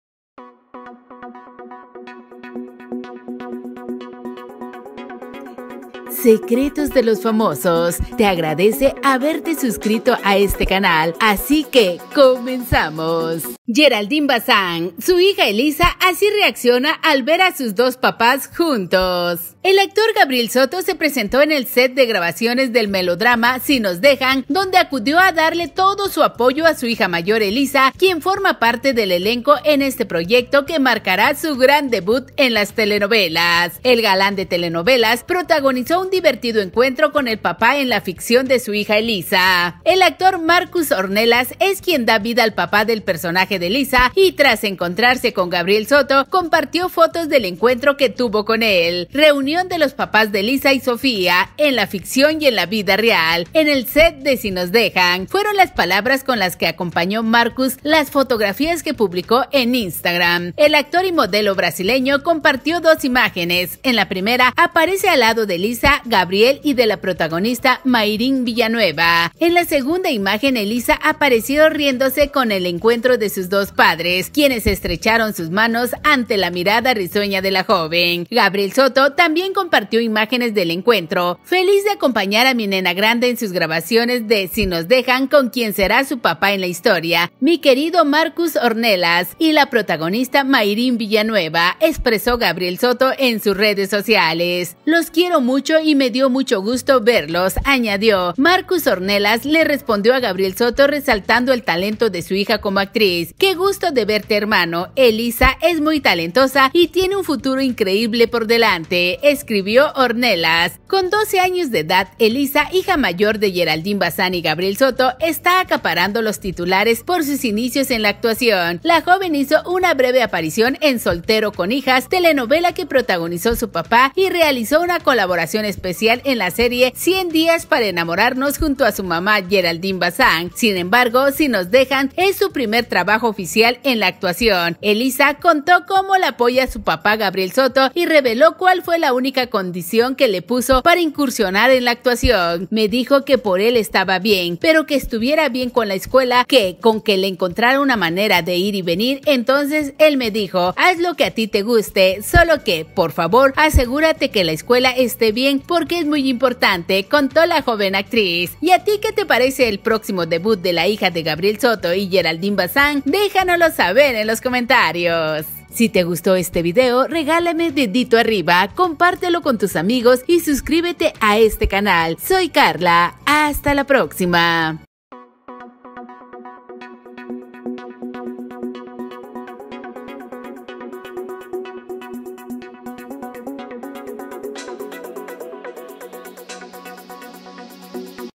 GERALDINE BAZÁN, SU HIJA ELISA ASÍ REACCIONA AL VER A SUS DOS PAPÁS JUNTOS
Te invitamos para que veas los demás videos que tenemos para ti